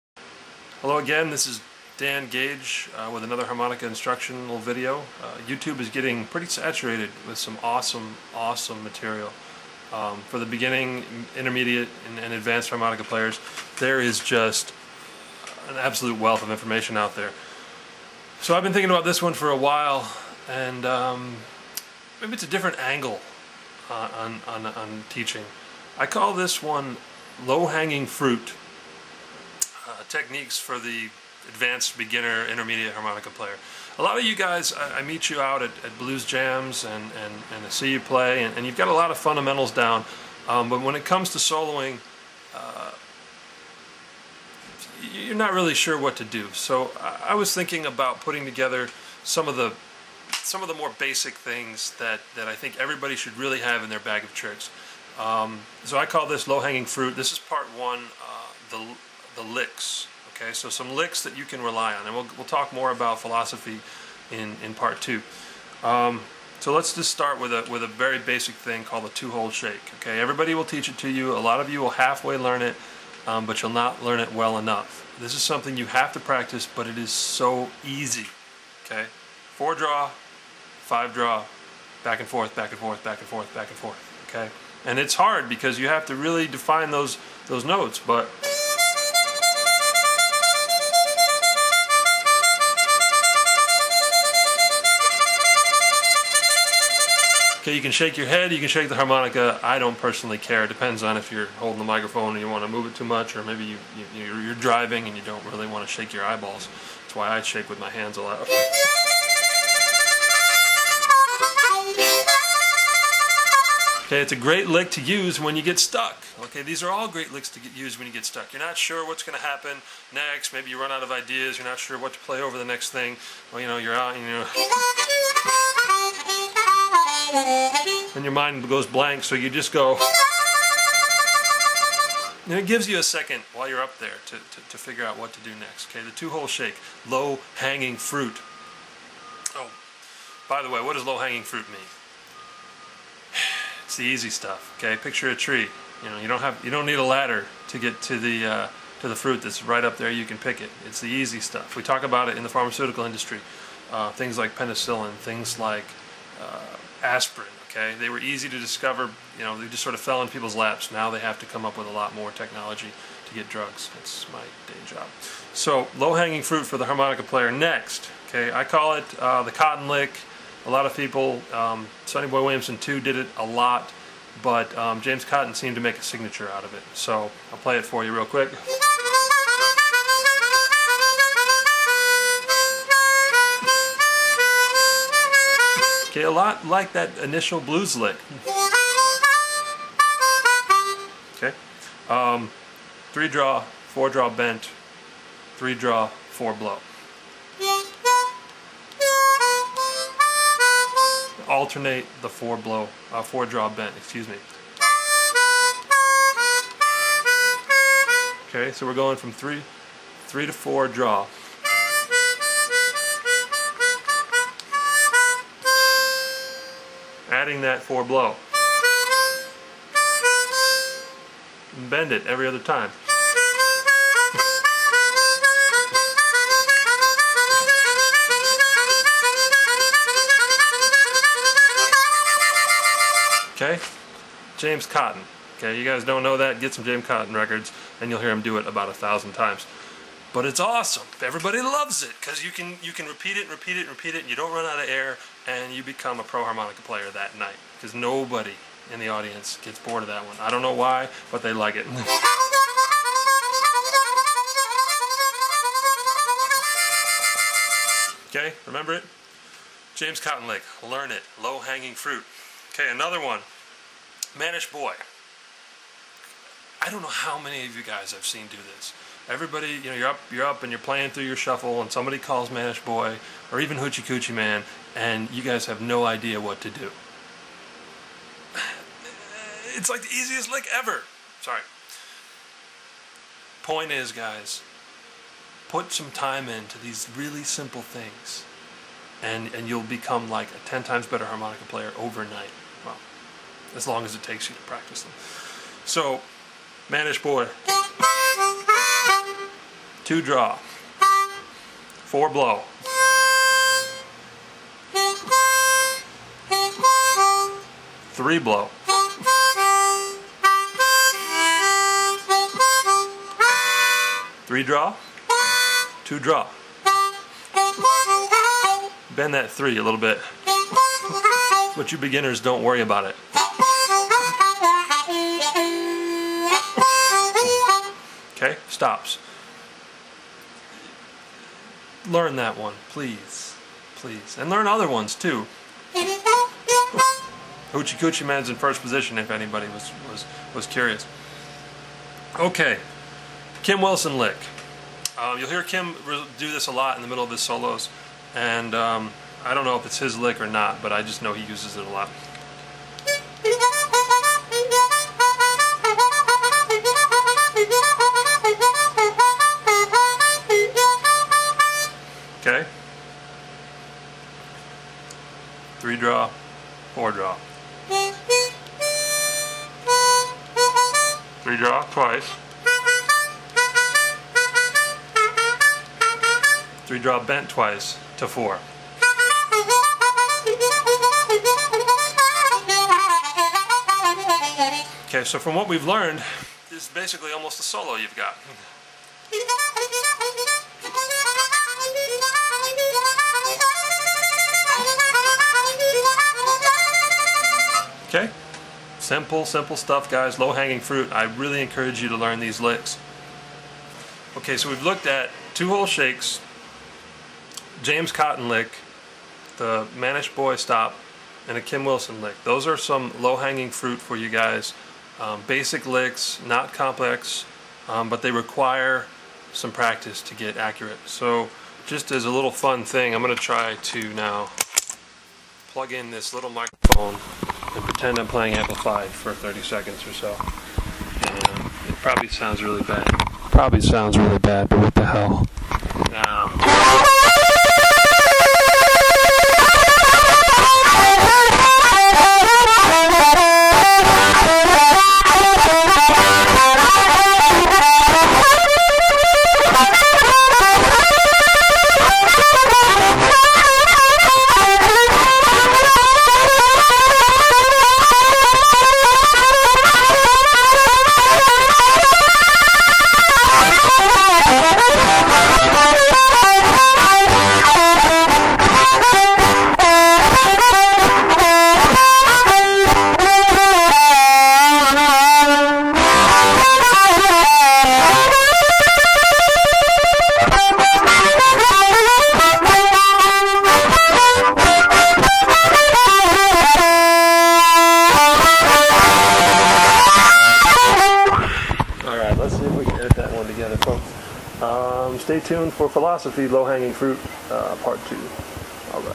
"Low Hanging Fruit" Part 1: Licks
"C" Harmonica
A video for any harmonica player, but directed specifically at advanced beginners and intermediates.  These are some easy licks for blues harpers to master.